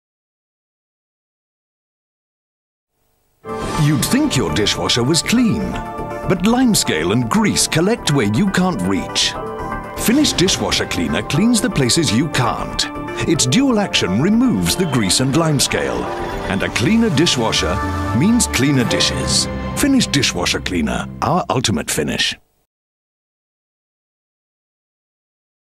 1998-1999: Finish Dishwasher Cleaner [Limescale & Grease]
Tittle: Limescale & Grease
Brand: Finish
Advertiser/Client: Reckitt
Agency: -
Media: TV Advert
Featuring: -
Guest Appearance: -
Voice Over: -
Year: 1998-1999
Location: -
Country: UK
Dishwasher: -
Model: -
Slogan: The Ultimate Clean.